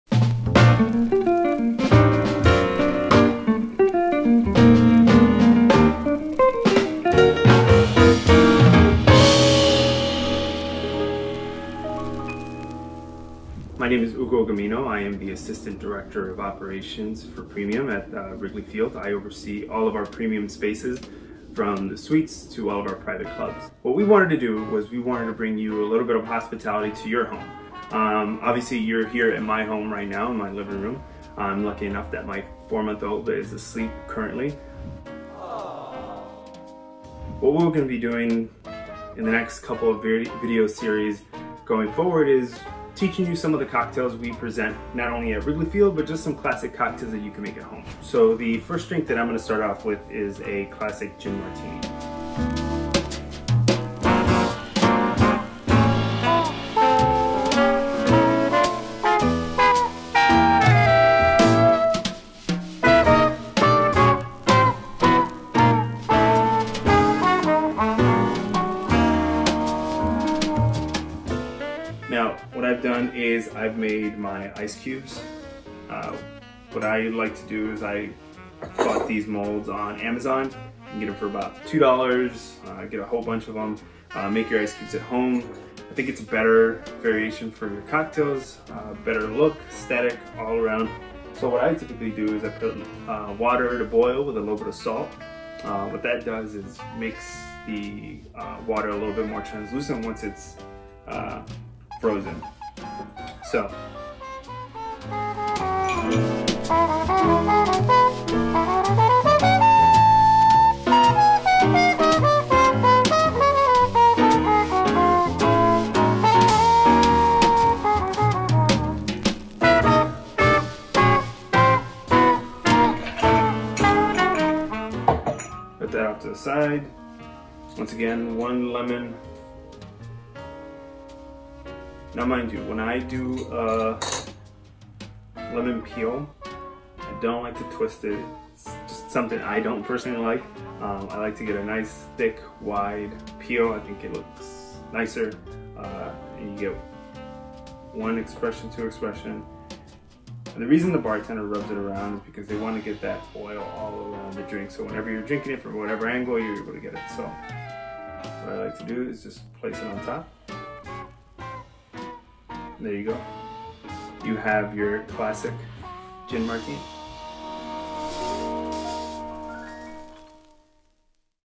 Cubs Cocktails at Home: Gin Martini
Featuring Levy Restaurants’ Assistant Director of Premium Hugo Gamino and Sipsmith Gin, this video will teach you how to make a classic Gin Martini served in Wrigley Field's premium spaces from your own home.

Must be 21 years of age or older to drink alcohol. Please drink responsibly.